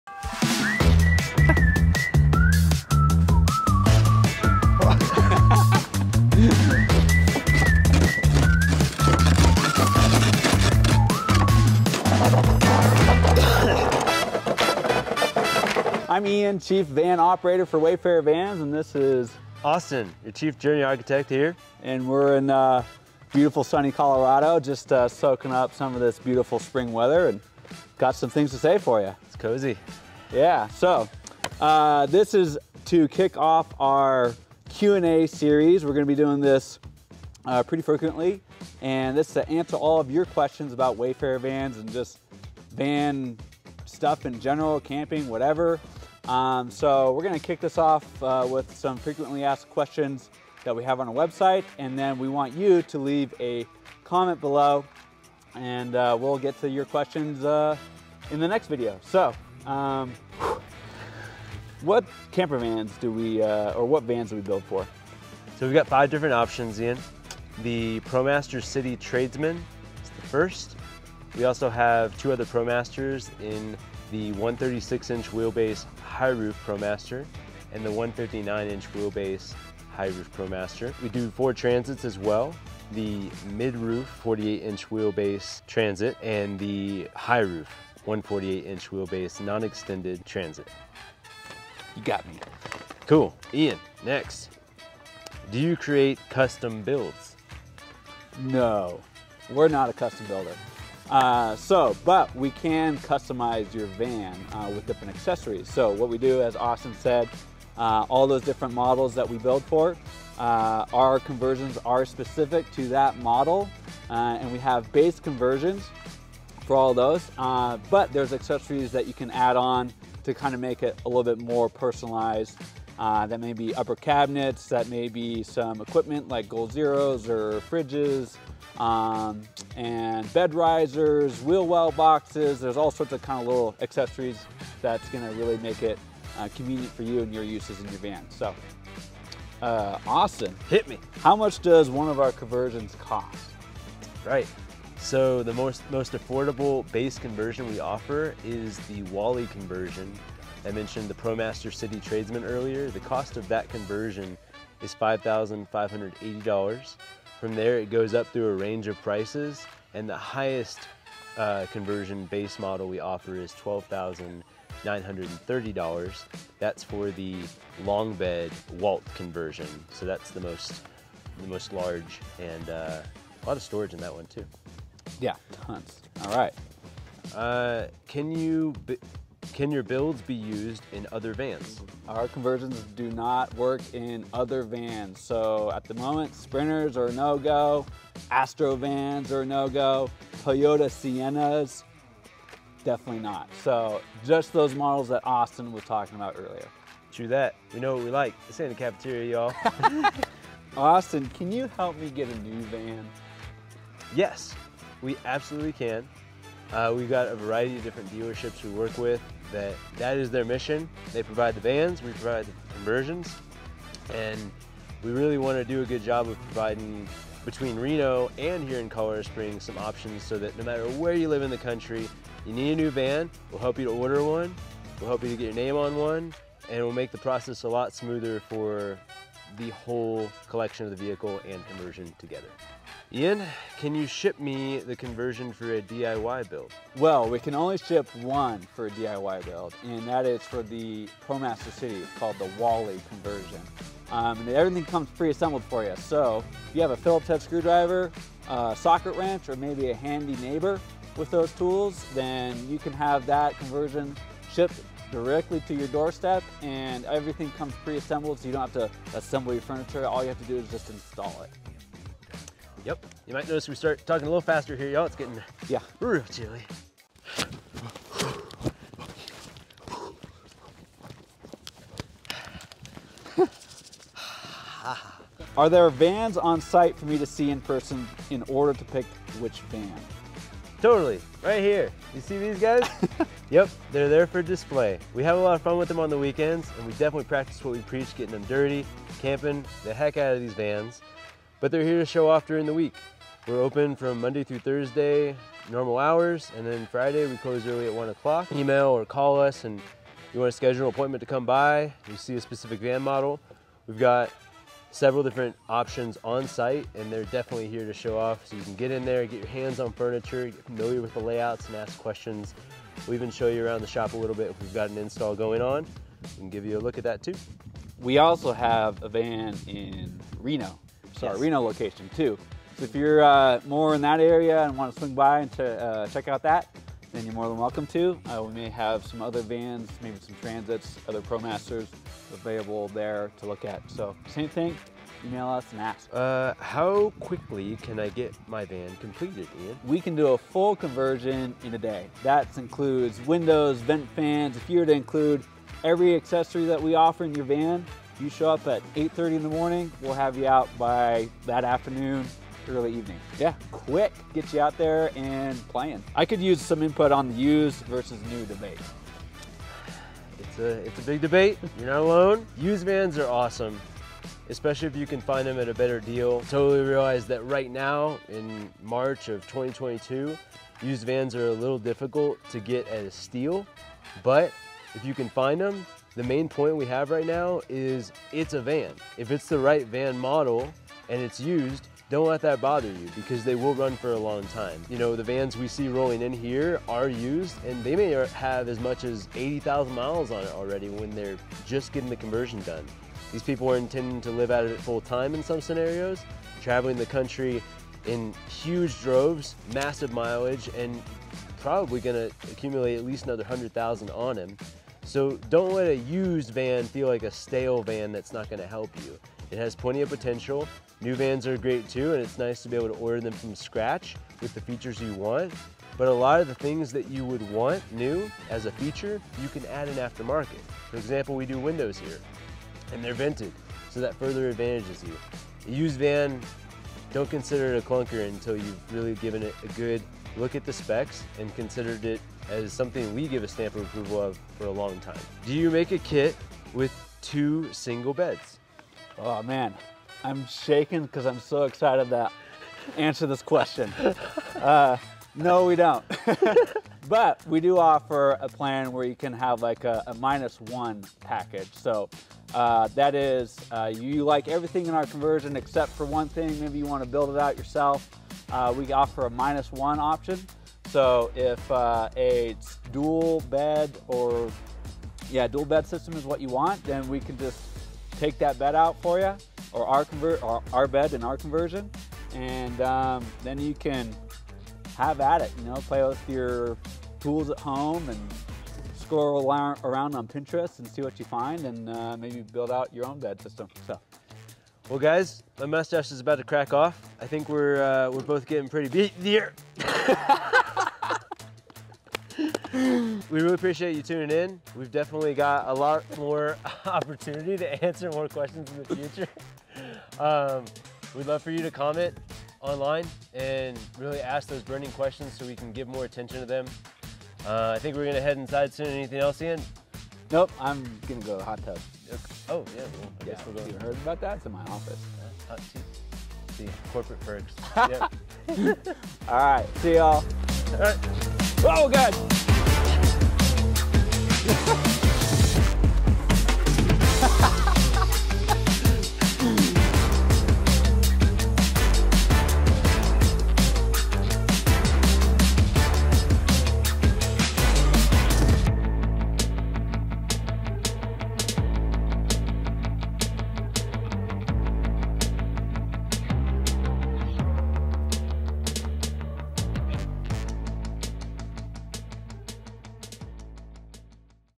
Q & A SERIES - Ep. 1: What Type of Vans Do You Create Conversions For?
On this cold spring day, we bring to you a new series we are trying out where we answer your most burning questions about Wayfarer Vans. Everything from details about our conversion vans to lifestyle questions or concerns you may have when considering getting a camper van.  Below you will see all the questions we tackle in this video and you can jump directly to any particular questions by clicking on the one you want to see.

If you have any questions of your own please leave them in the comments below and we will be sure to add them to future Q and A videos!

0:00 - Intro
1:00 - Which vans do you create conversions for?
1:30 - Do you create custom builds for vans?
2:20 - How much does a conversion and van cost?
2:57 - Can your builds be used in other vans?
3:20 - Can you help me get a new van?
3:52 - Can you ship me the conversion for a DIY build?
4:34 - Are there conversion vans on sight for me to see in person in order to pick which van?
5:34 - How quickly can I get my conversion van completed?
5:55 - I could use input on the used vs. new debate.
7:15 - Do you make a camper van kit with 2 single beds?